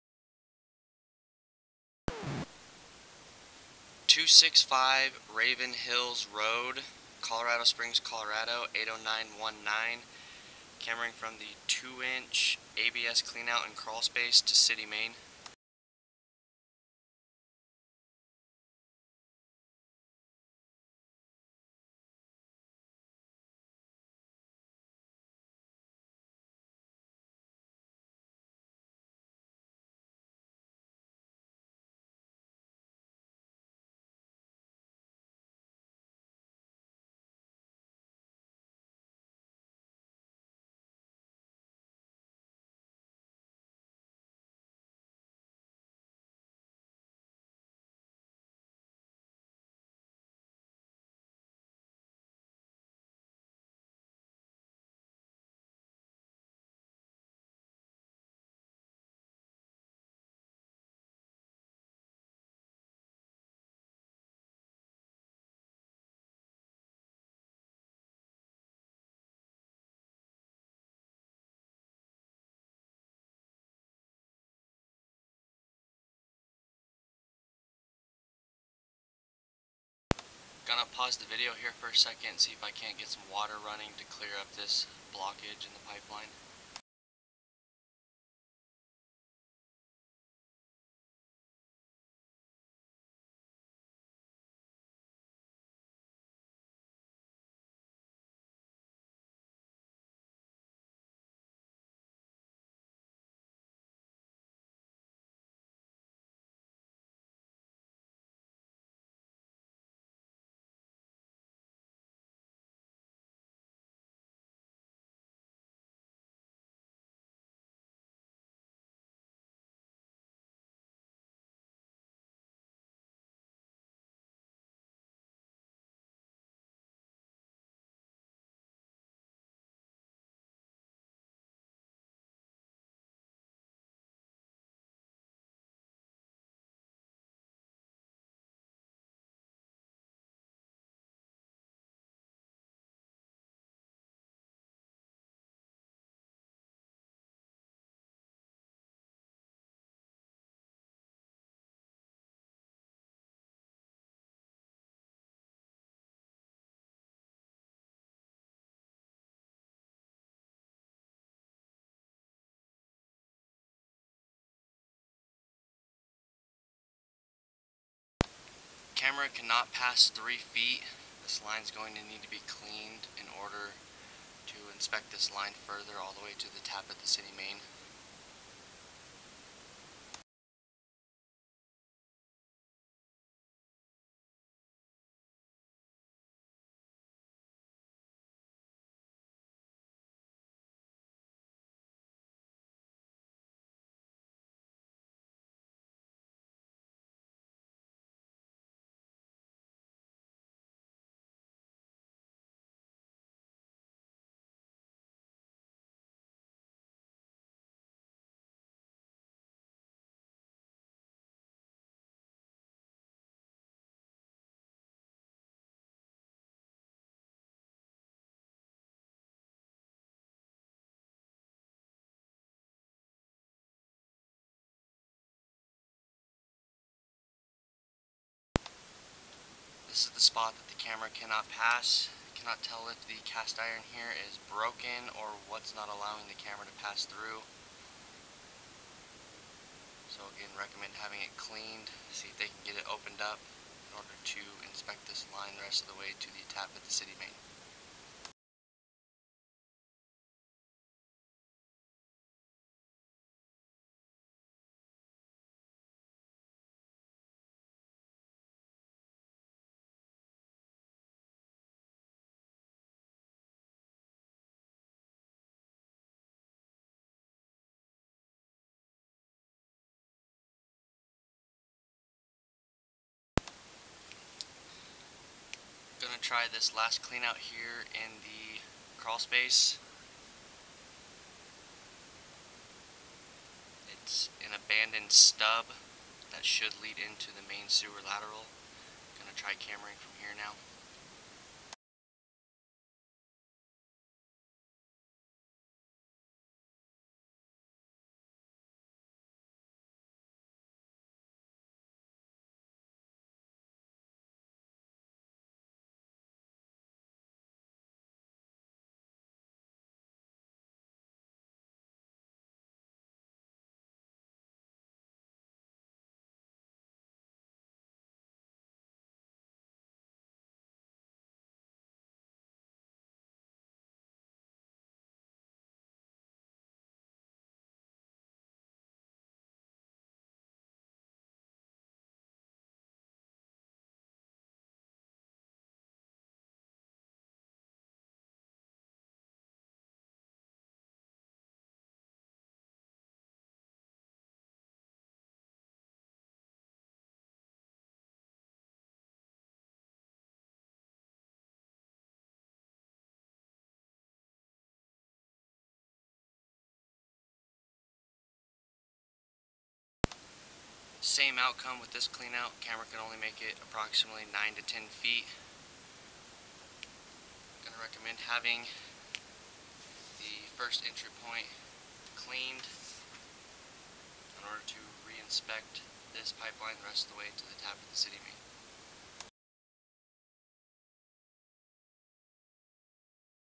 SEWER SCOPE 6/01/2023 265 Raven Hills Drive Colorado Springs, CO 80911
REPORT DETAILS:
ENTRY POINT:  SIZE, MATERIAL & LOCATION
- 1st entry point 2 in. ABS PVC clean-out in crawl space
- 2nd entry point 2 in. SCH 40 PVC/ cast-iron stub clean-out in crawl space

DISTANCE TO ROOF VENT
- N/A.

EXTERIOR TRANSITION: DISTANCE SIZE & MATERIAL
- N/A
- N/A

ALL TRANSITION (S) W/ FOOTAGES:

From 1st entry point 00-0.5 ft. ABS
- 0.5-01 ft. SCH 40 PVC
- 01-03 cast-iron

From 2nd entry point 00-0.5 ft.  SCH 40 PVC
- 0.5-09 ft. cast-iron

DISTANCE TO THE TAP AT THE CITY MAIN
- N/A

SIZE AND MATERIAL OF THE TAP AT THE CITY MAIN
- N/A

ISSUES, RECOMMENDATIONS & OR OTHER DETAILS
- During the time of inspection the camera came to a dead stop at 3 ft. from the 1st entry point  and 9 ft. from the second entry point
- In order to have this line properly inspected to the tap at the city main, is to have these two spots cleaned as well as having the time cleaned from these 2 spots all the way to the city main to get a proper inspection. it is unclear of why the camera came to a stop in both spots due to the grease making the camera lens go black. In all, the line needs to be cleaned and or fixed if damaged at these two spots in order to finish the inspection to the city main.